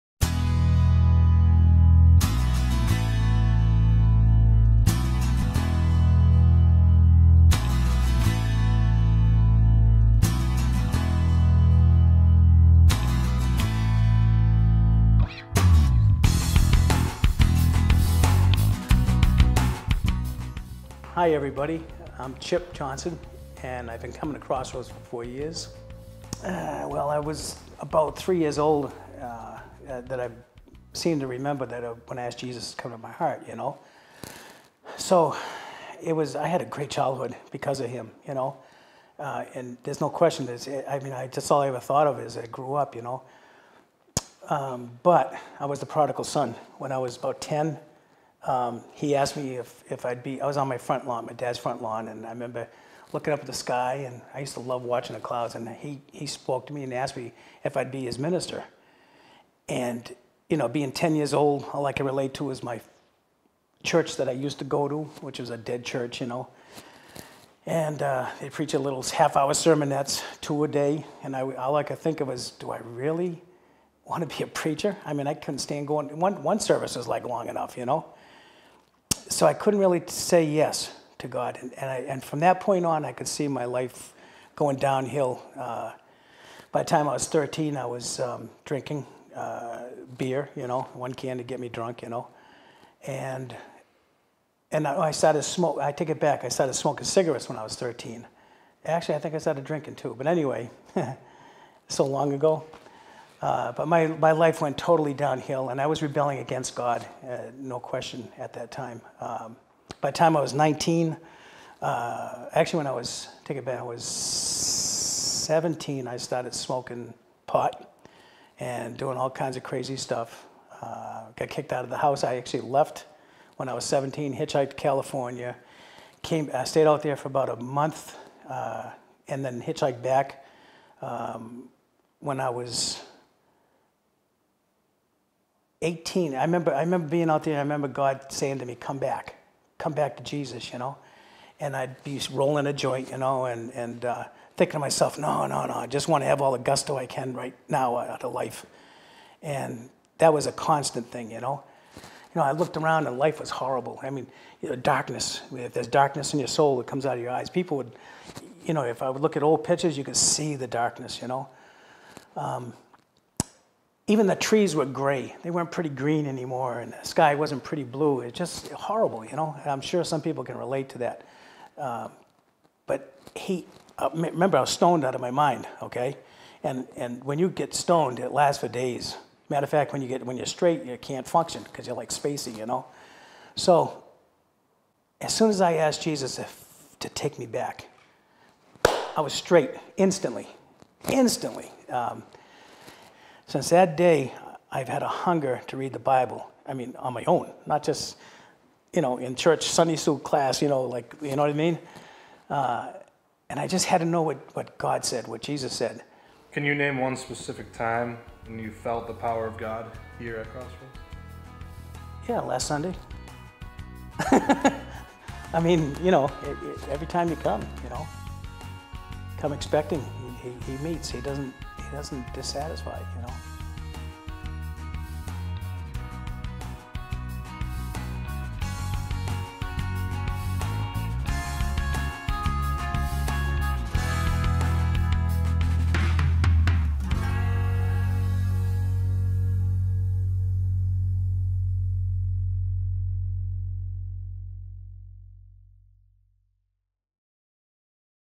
Stories - Chip's Story
Everyone has a story to tell.  But the greatest story is that of a changed lives, changing lives.  Chip Johnson shares his testimony of a wild youth and how God reached out to him and pulled him back. What the devil meant for destruction and God turned around for His glory.  Join us Sundays at 10am for more "Stories" and watch our sermons online at CrossroadsConnects.com